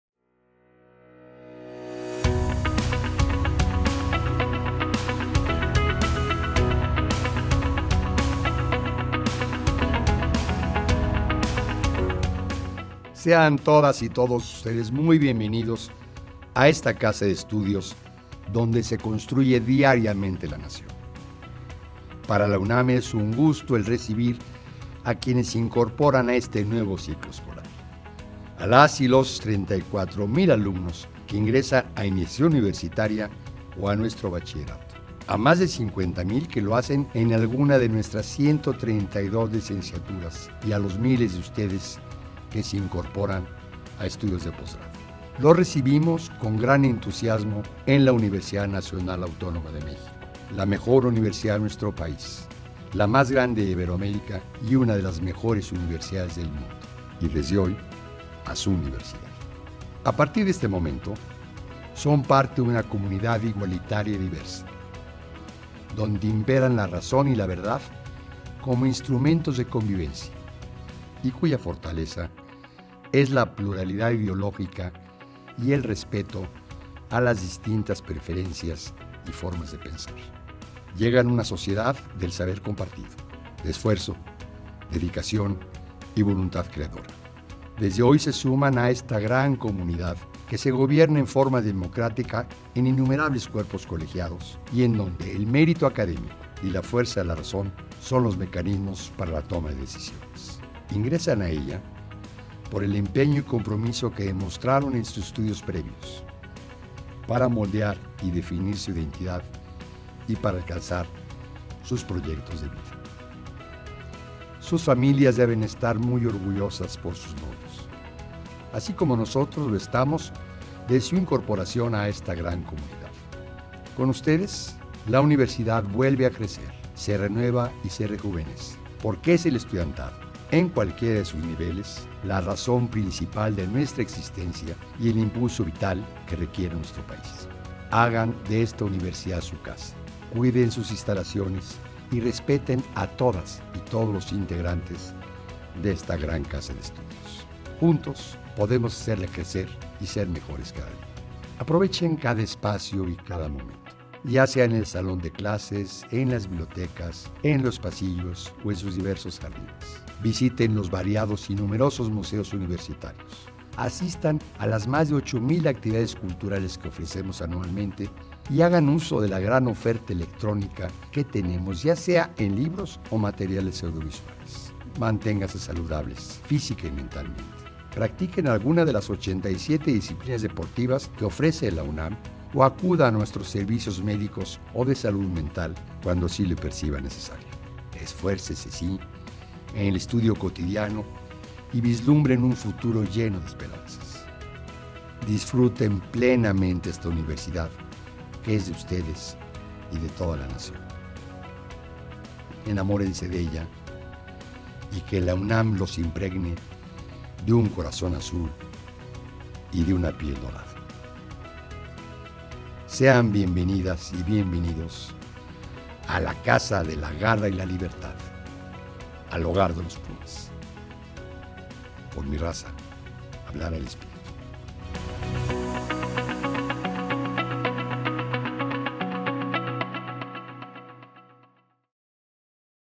Bienvenida del Rector UNAM a alumnas y alumnos de nuevo ingreso al ciclio escolar 2022-2023...
El Rector de la UNAM, Dr. Enrique Graue te da la más cordial bienvenida al ciclo escolar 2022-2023